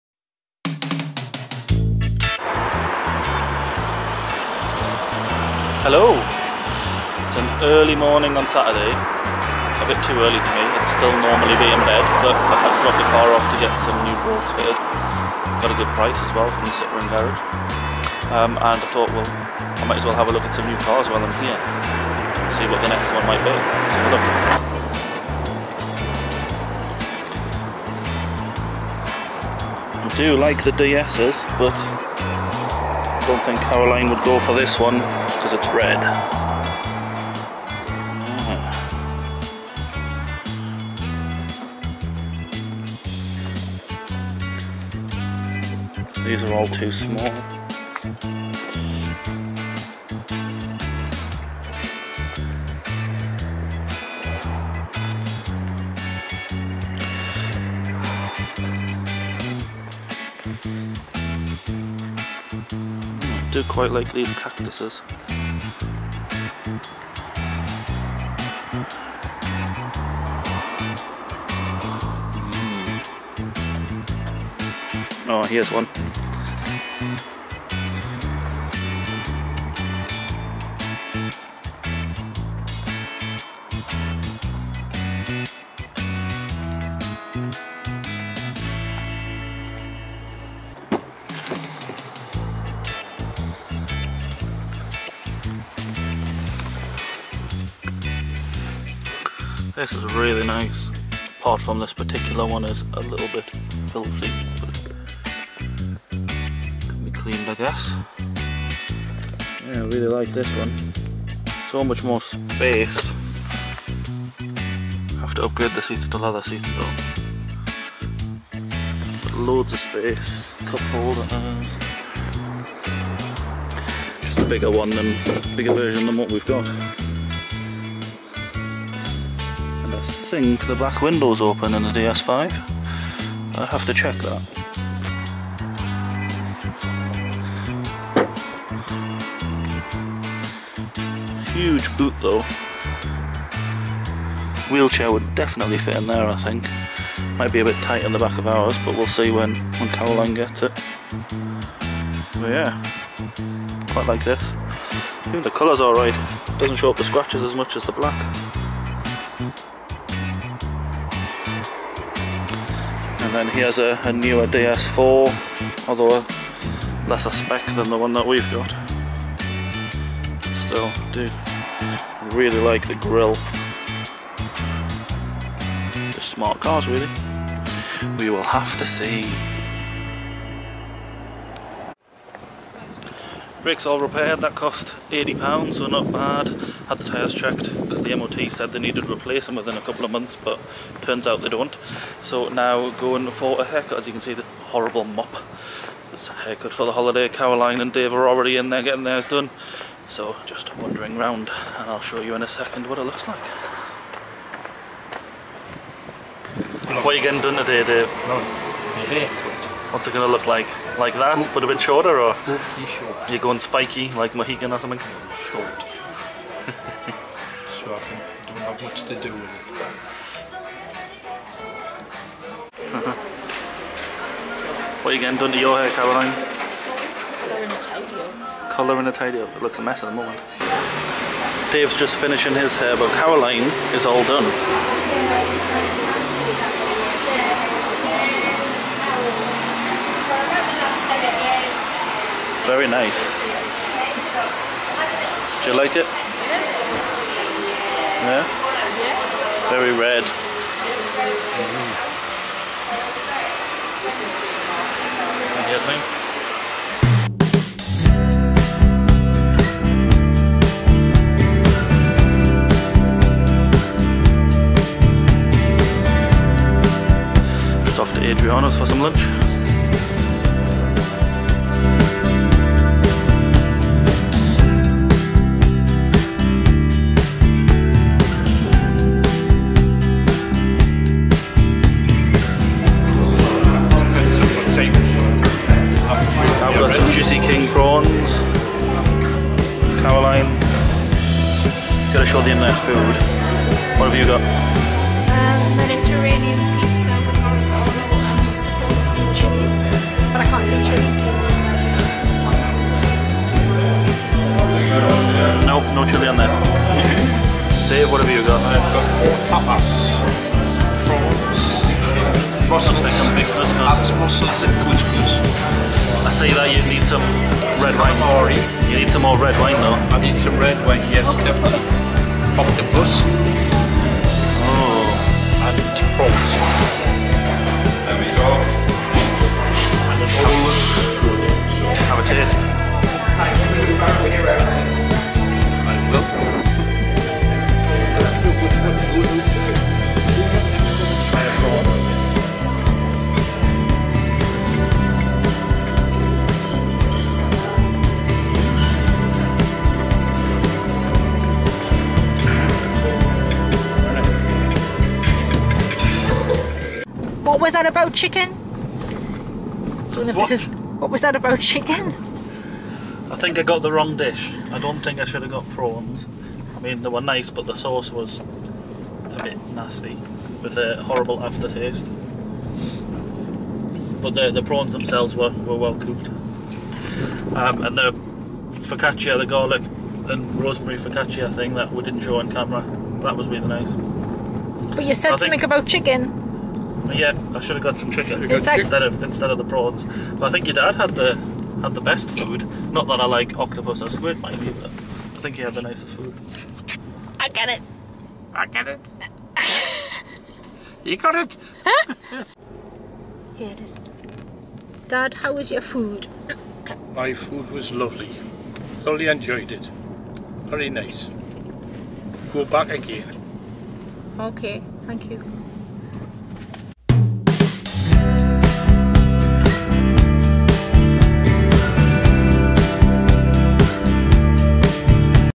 Cars, haircuts and a meal at Adriano's Gosforth
In this weekly vlog, I take the car in for some repairs, check out what we might upgrade to in the future and then we all go for a haircut. We then have a meal at Adriano's.